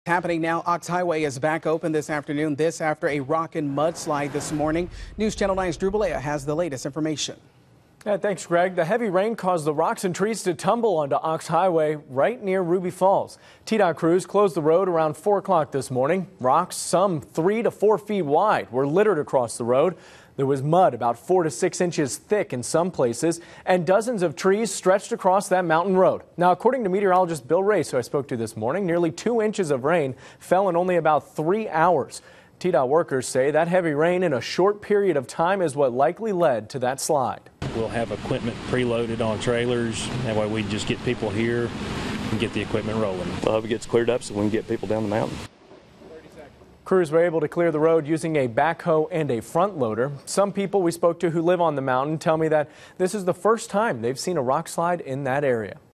UPDATE: Ochs Highway Now Open After Mudslide
UPDATE: Ochs Highway on Lookout Mountain is now open after a morning rock slide forced it to close.

Nearly two inches of rain fell in a three hour period from 2:00 to 5:00am. It caused rocks, mud and trees to spill over Ochs Highway on Lookout Mountain.

"It's the first mudslide I've ever seen, so it is pretty shocking," said David Bennett, who lives on Lookout Mountain.

"They just happen, some times they are massive rock slides like on [highway] 64, other times they're just a pile of rocks," said Chris Smith with the Tennessee Department of Transportation.

He surveyed  the closed road.

"Make sure nothing else was going to fall down on top of us," said Smith.

Rocks, three to four feet wide, trees, and mud inches thick covered a 30 foot section of the road.

"I thought maybe there was a branch down or something and there wasn't a sign so I wasn't sure what was going on," said James Barnett who was trying to get to work.

Dozens of cars were forced to turn around after going around T-DOT barriers.
Smith says they're ready for days like today.

"We'll have equipment pre loaded on trailers that way we can just get people here and get the equipment rolling," said Smith.

Flash flooding caused other road closures in Downtown Chattanooga.

On East 20th Street one car got stuck in nearly three feet of water, while others pushed through.

Cummings highway also flooded near the I-24 exit.

While some crews dealt with flood waters, others had to wait until daylight to move the rocks and trees.

"I hope they get it cleared up and we can get people down the mountain.," said Bennett.

TDOT crews spent a few hours clearing the road, and were able to get it back open just before noon.

By Drew Bollea